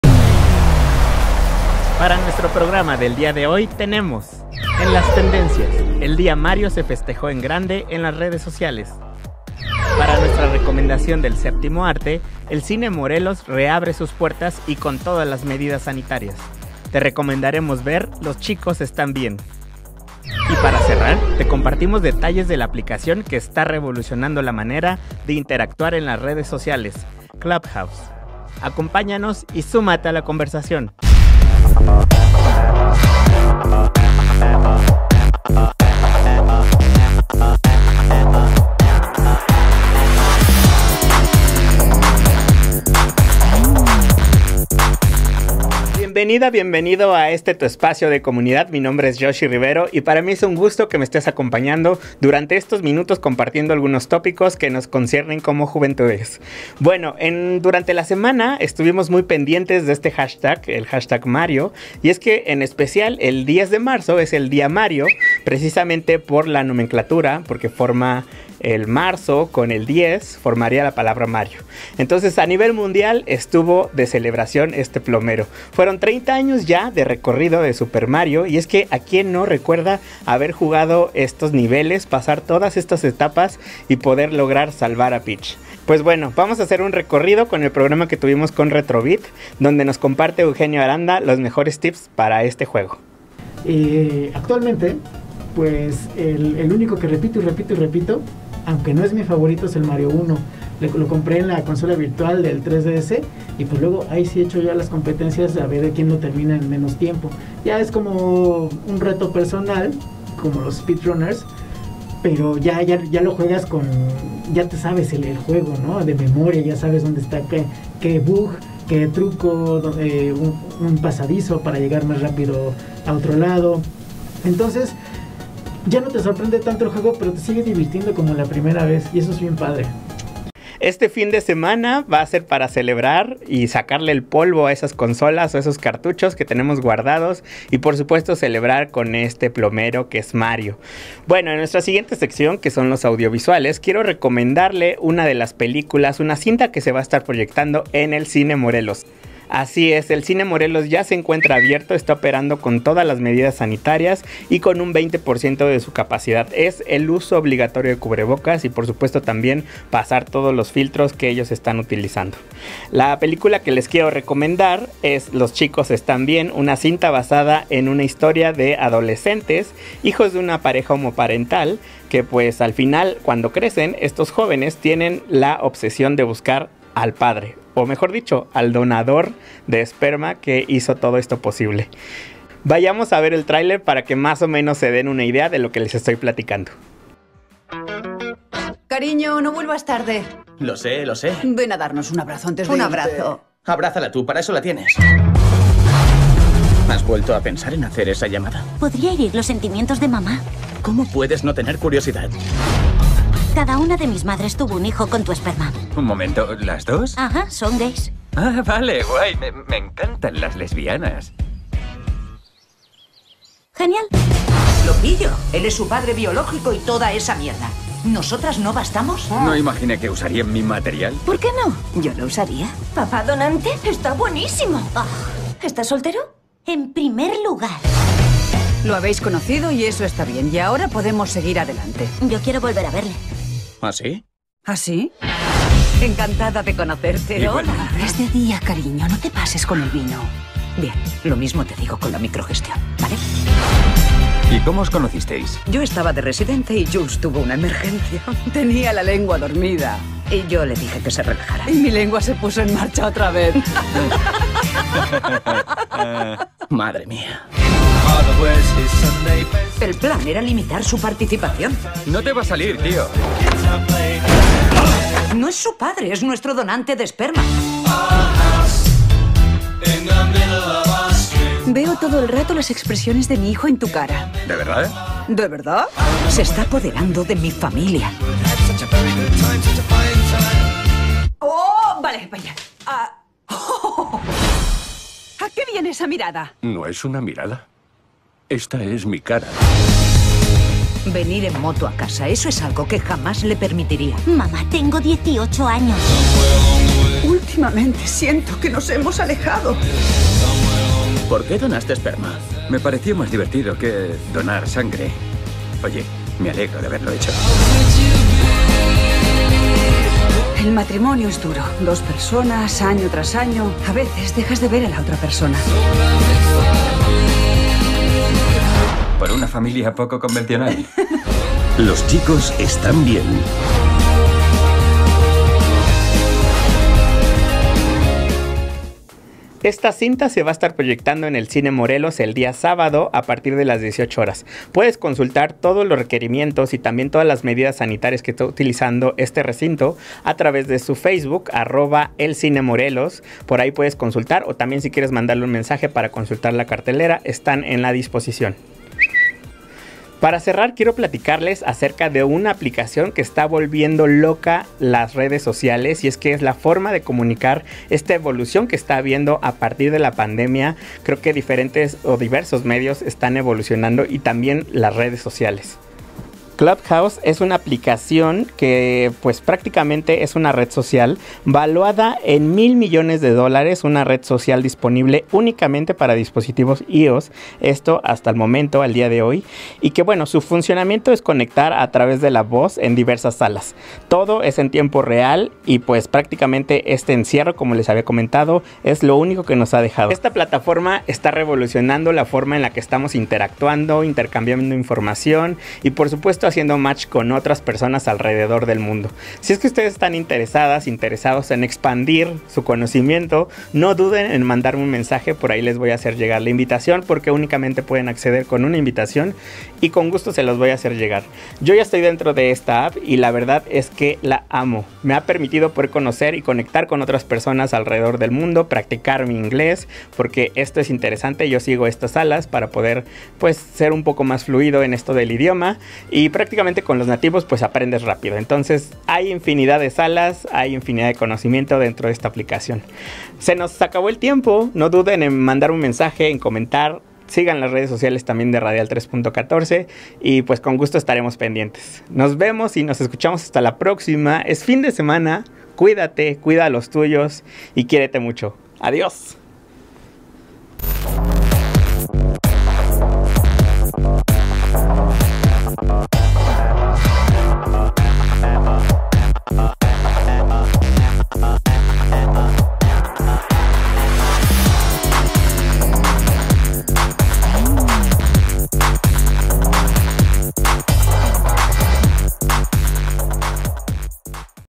Comunidad 12/03/21
Para nuestro programa del día de hoy tenemos:
- El Cine Morelos reabre sus puertas y aquí te recomendamos un filme